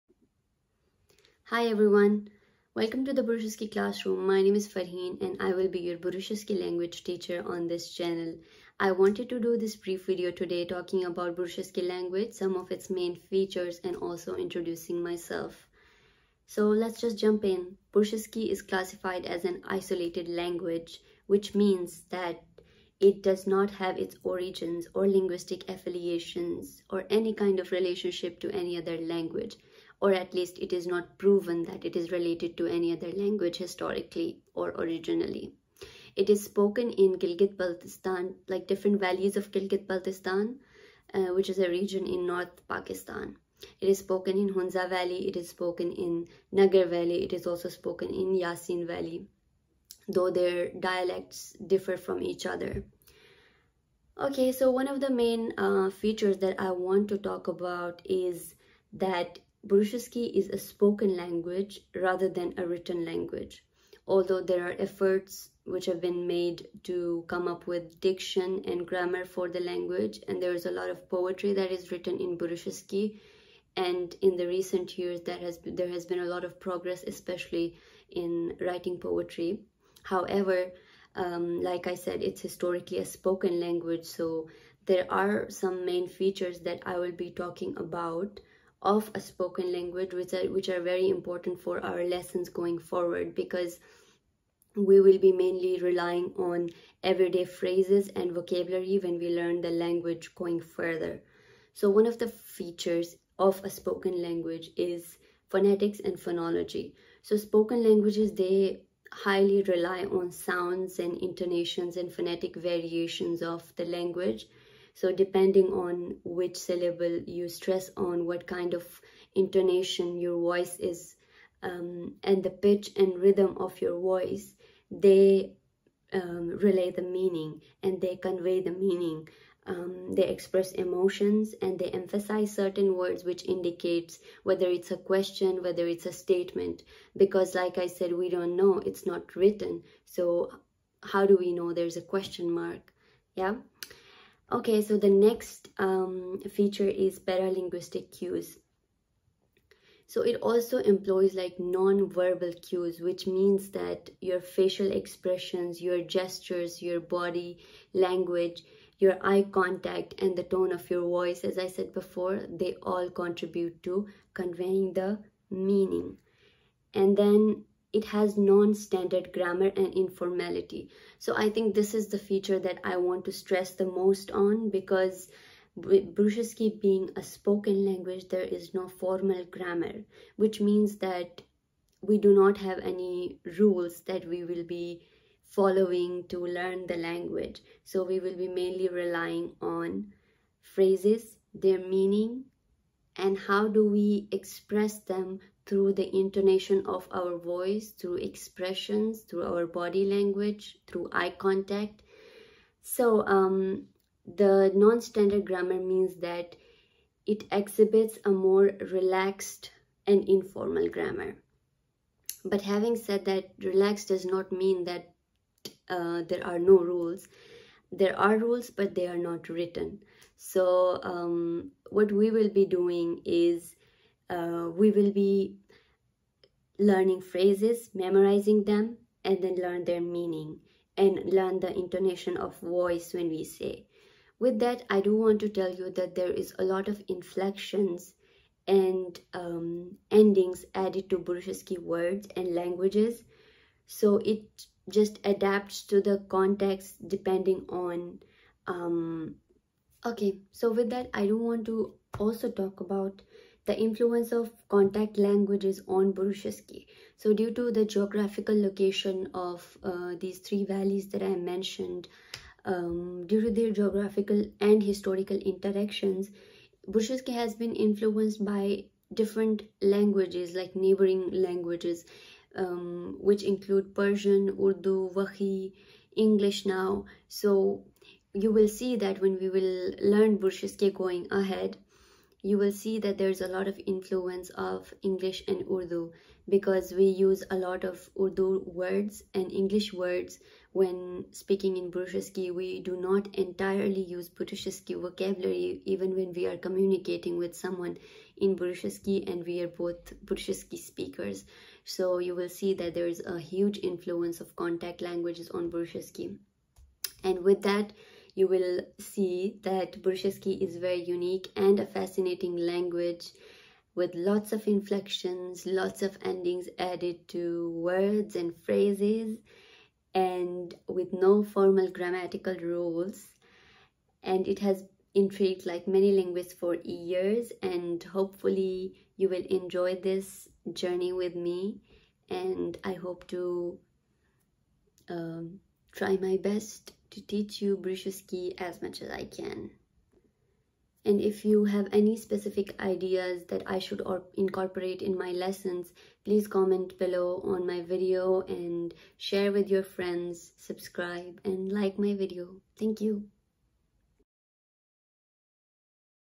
Introduction | The Burushaski Classroom
This is a brief introduction to Burushaski language and myself. I will be posting lessons soon.